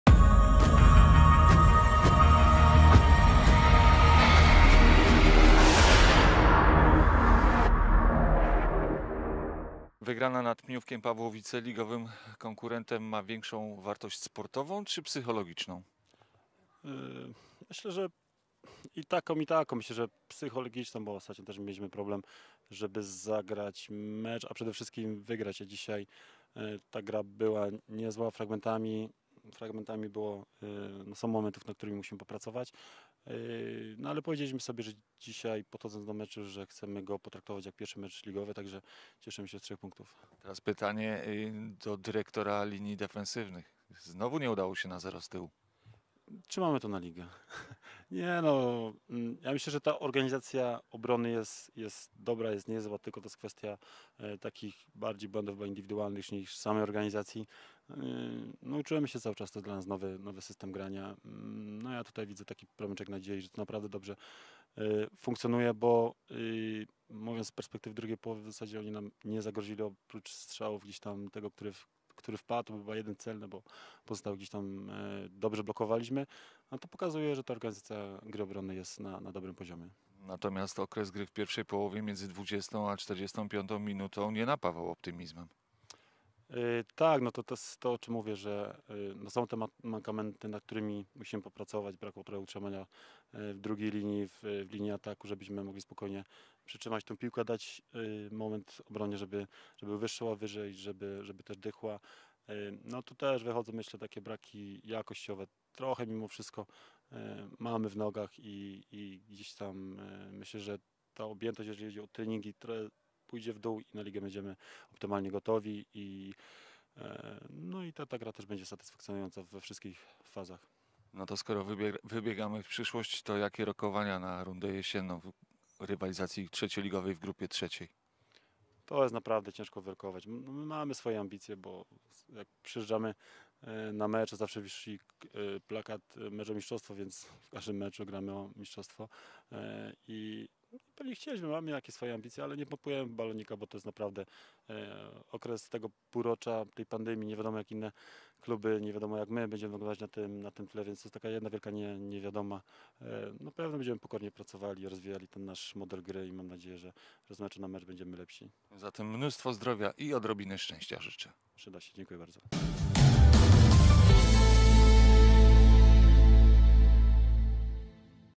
Rekord - Pniówek Pawłowice 3:1 (0:0)
Ostatni sparing naszych trzecioligowców przed startem ligowych rozgrywek.